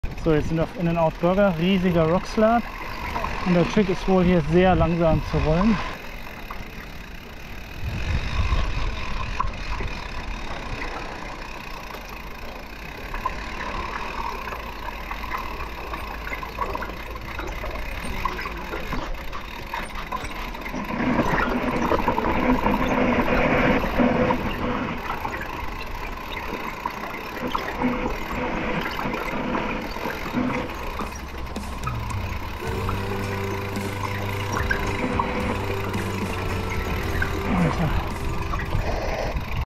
SQUAMISH BC 🍁 Biggest & Slowest Rock Slab 🤩
Squamish, British Columbia, Canada. In-n-Out Burger Trail, July 2023

Riders:
Jay Rides Trails (Cam)
@TrailTrackerSwitzerland (Slowly Shreds)